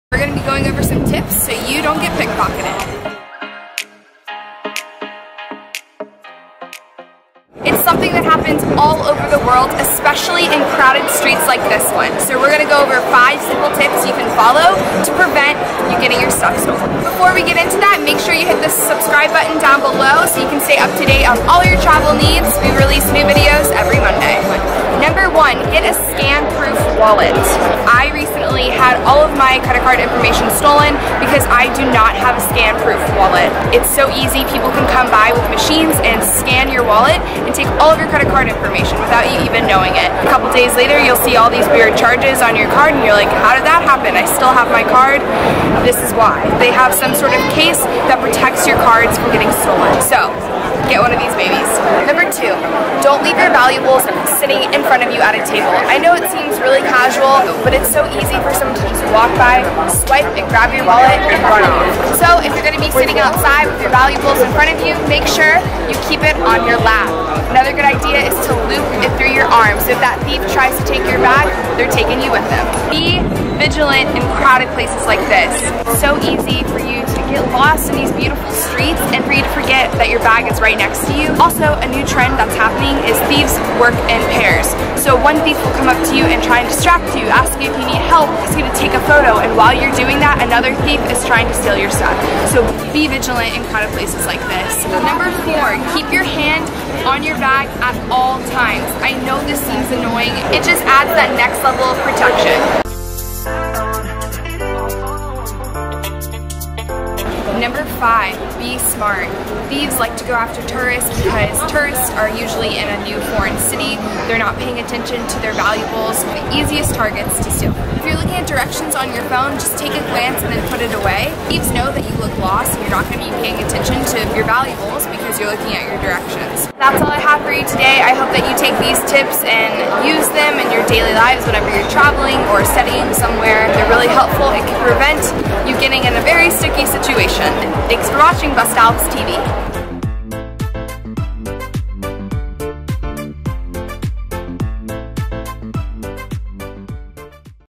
5 Tips To Prevent Pick-pocketing
Pick-pocketing happens everywhere but especially in crowded touristy cities. Bus2alps has 5 simple tips you can follow to help you avoid getting your thing stolen.